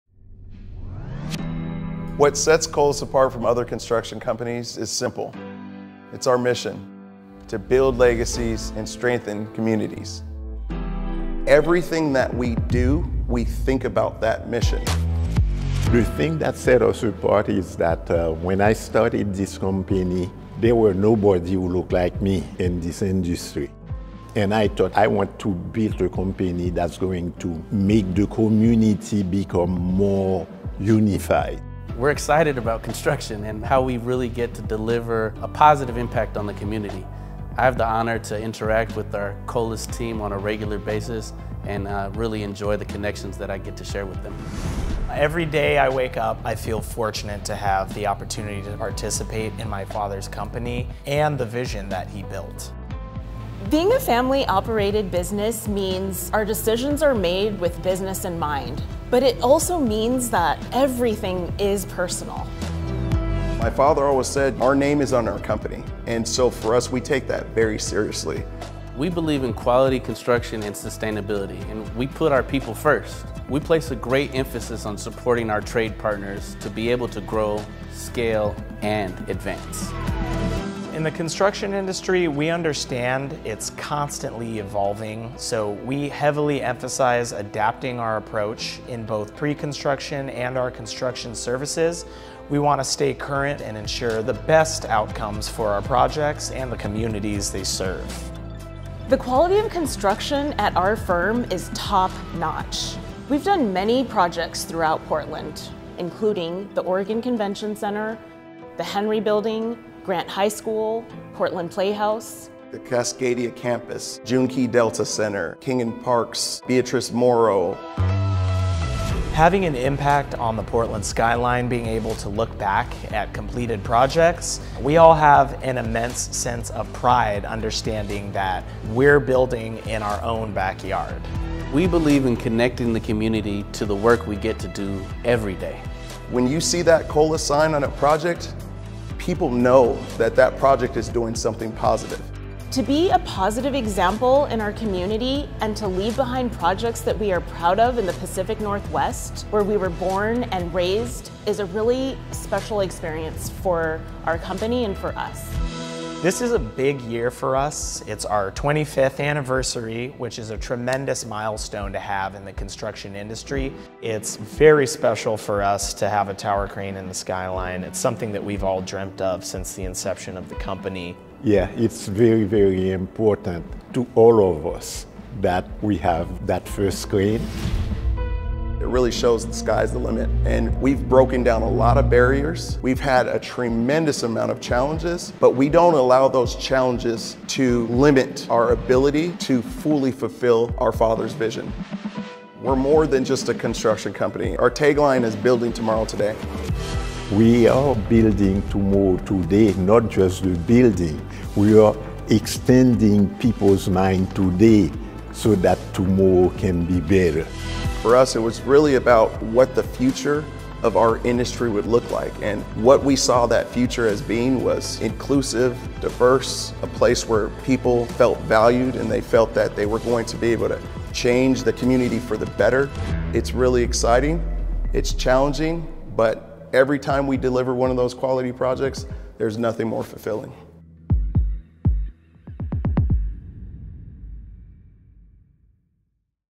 Celebrating the 25th Anniversary of Colas Construction
FULL VIDEO: Learn about the 25th Anniversary year with COLAS Founder Hermann Colas, Jr., and the second generation of @colasconstruction as they share about excellence and joy while reflecting on their commitment to Building Tomorrow, Today!

COLASCONSTRUCTION.COM